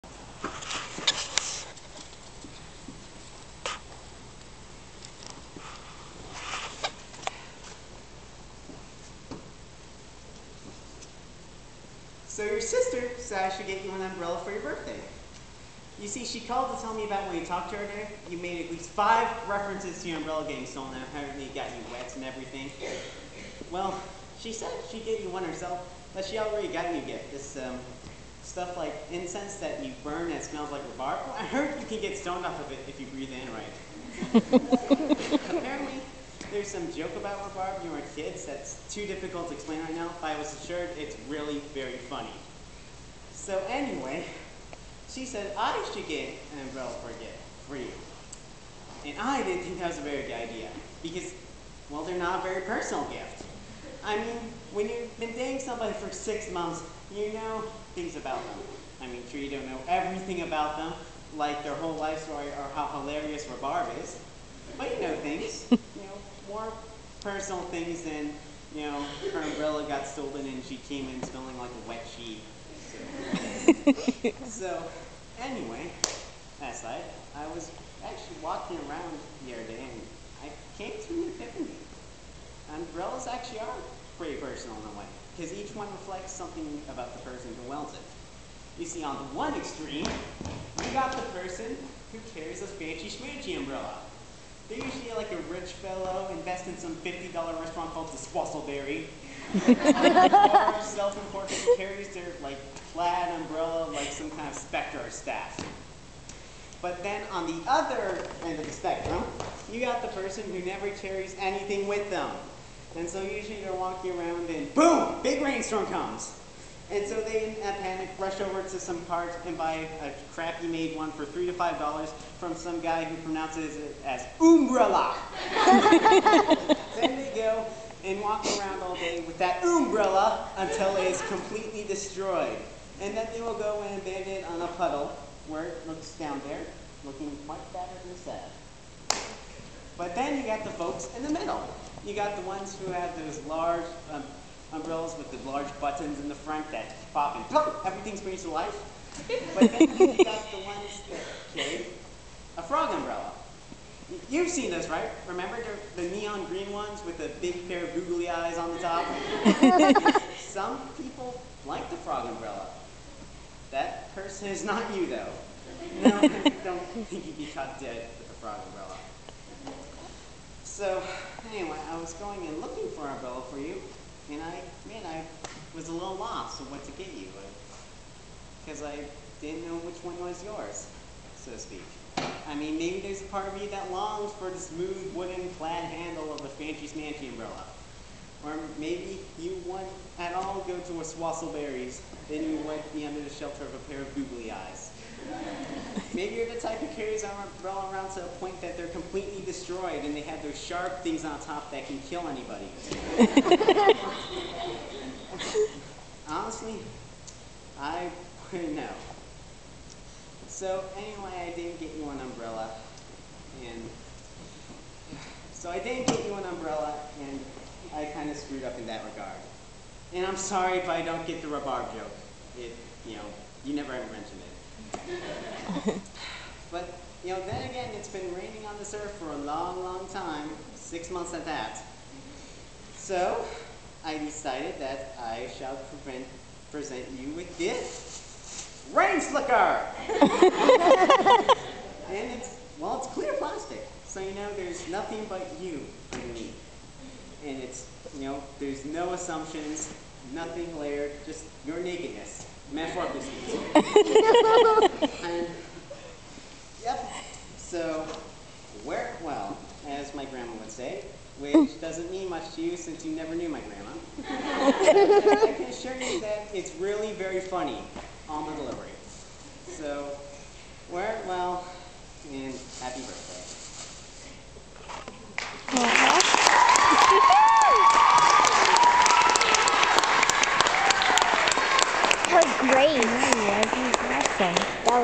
The Umbrella Monologue- Joshua Foley 2008
This is a comedic monologue performed by my son Josh as part of his high school drama final. I am sharing this not only because I think he is great in it, but because he has had so many challenges in his young life, and I am so very proud of him.